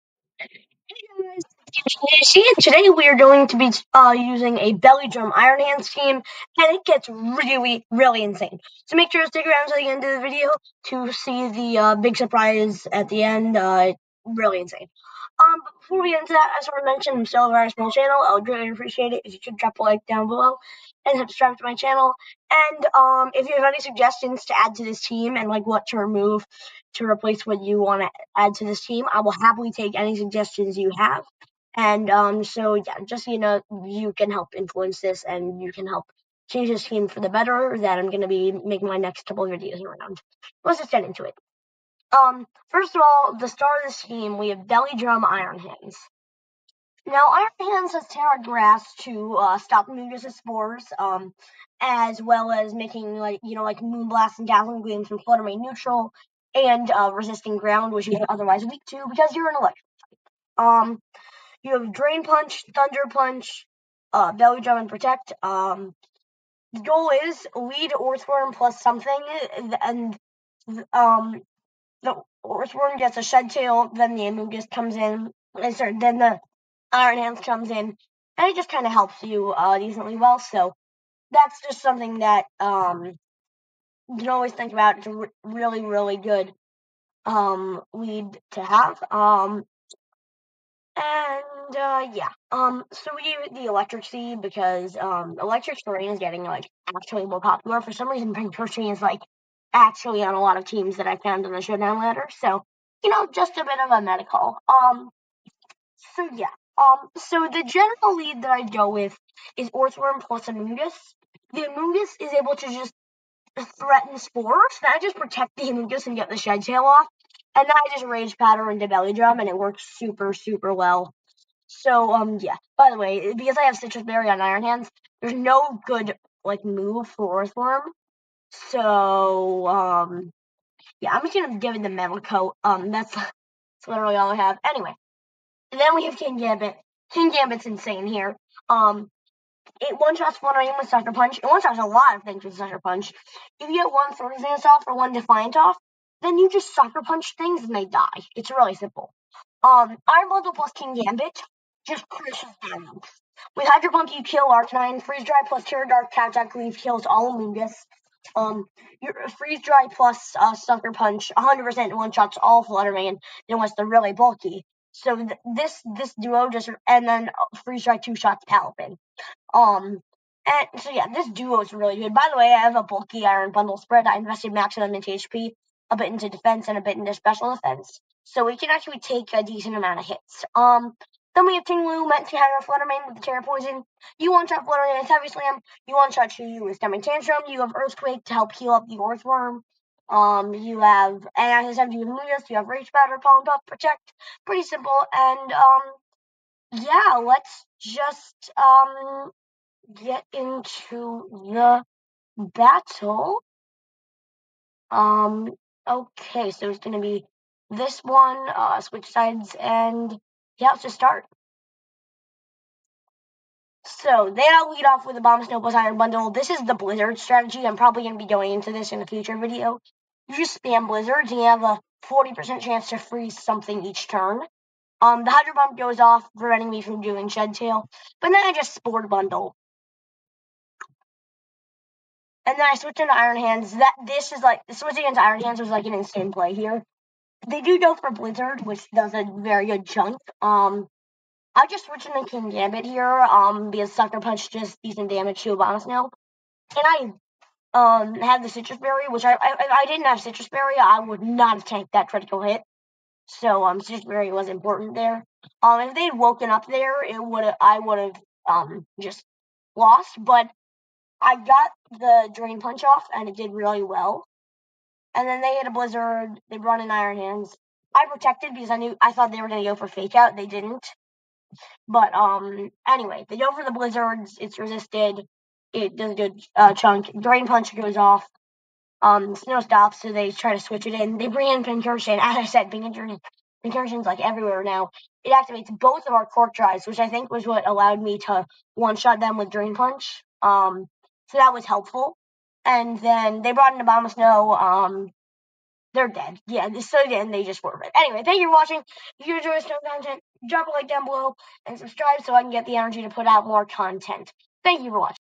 Belly drum iron hands sweep
in this video i will be showing a Belly drum iron hands sweep that you can replicate with this team

Paste: meta counter (Iron Bundle) @ Focus Sash
Ability: Quark Drive
Level: 50
Tera Type: Ghost
EVs: 252 HP / 36 Def / 4 SpA / 124 SpD / 92 Spe
Modest Nature
IVs: 0 Atk
- Freeze-Dry
- Hydro Pump
- Encore
- Protect

Amongus (Kingambit) @ Black Glasses
Ability: Defiant
Level: 50
Tera Type: Dark
EVs: 4 HP / 252 Atk / 4 Def / 228 SpD / 20 Spe
Adamant Nature
- Kowtow Cleave
- Sucker Punch
- Swords Dance
- Protect

Iron Hands @ Sitrus Berry
Ability: Quark Drive
Level: 50
Tera Type: Grass
EVs: 212 HP / 20 Atk / 4 Def / 252 SpD / 20 Spe
Careful Nature
- Drain Punch
- Thunder Punch
- Belly Drum
- Protect

hat deer (Ting-Lu) @ Assault Vest
Ability: Vessel of Ruin
Level: 50
Tera Type: Poison
EVs: 4 HP / 196 Atk / 12 Def / 252 SpD / 44 Spe
Adamant Nature
- Fissure
- Heavy Slam
- Stomping Tantrum
- Earthquake

Amoonguss @ Mental Herb
Ability: Regenerator
Level: 50
Tera Type: Water
EVs: 244 HP / 100 Def / 164 SpD
Relaxed Nature
IVs: 0 Atk / 0 Spe
- Spore
- Rage Powder
- Pollen Puff
- Protect

Orthworm @ Metal Coat
Ability: Earth Eater
Level: 50
Tera Type: Water
EVs: 244 HP / 12 Def / 252 SpD
Careful Nature
- Body Press
- Shed Tail
- Heavy Slam
- Iron Defense